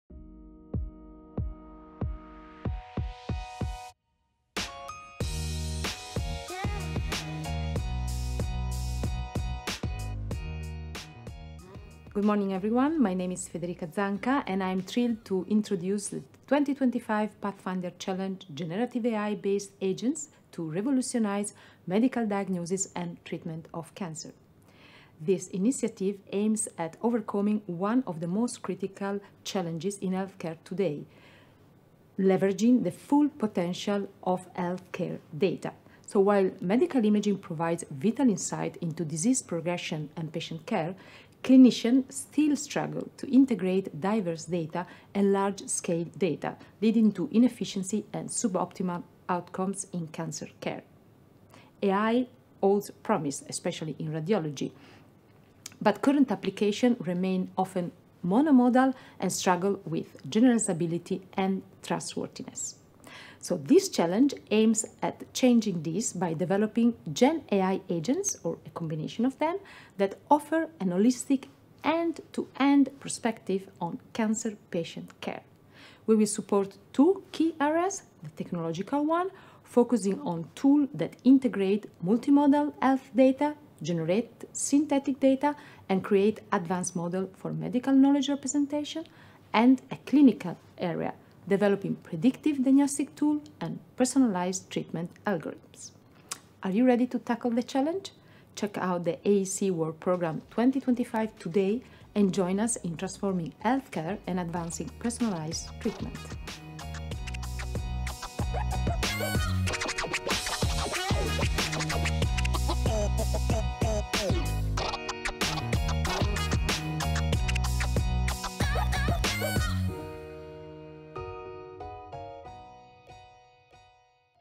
Generative-AI based Agents to Revolutionize Medical Diagnosis and Treatment of Cancer
Federica Zanca, EIC Programme Manager for Medical imaging and AI in healthcare, on EIC Pathfinder Challenge 2025: Generative-AI based Agents to Revolutionize Medical Diagnosis and Treatment of Cancer.

Are you ready to tackle the challenge?
Call opens in spring 2025 until 29 October 2025.